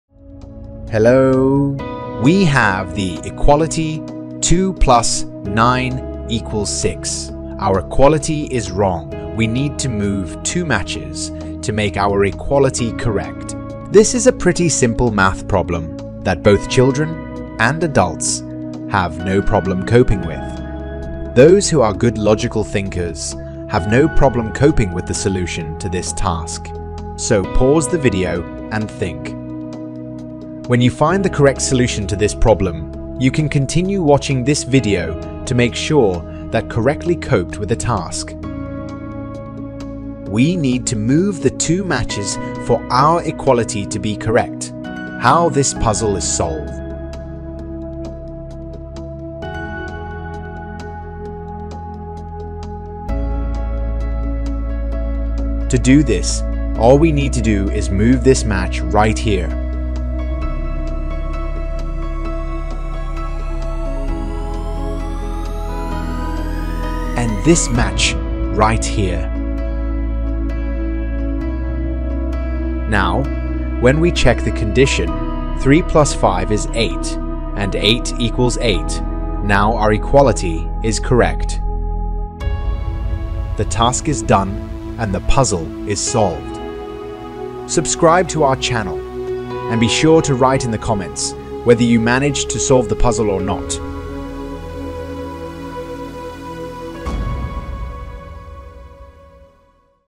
Match puzzle - move 2 matches to make equation correct | Riddles with matchstick 2+9=6
Match puzzle 2+9=6. We need to move two matches to make the equality true.
Do you know how to do that? Did you manage to solve the matchstick puzzle 2+9=6?

00:00 Match puzzle equation
00:53 Answer to matchstick puzzle